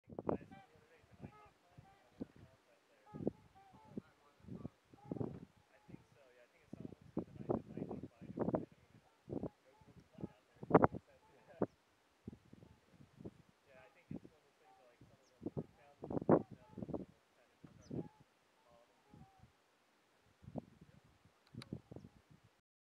Animals I've Found Episode 14 - Trumpeter Swan
Found in a field in southern Illinois. There were around 50 trumpeters and 3 tundra swans. There was also a very tiny Ross's goose that was within the group of swans.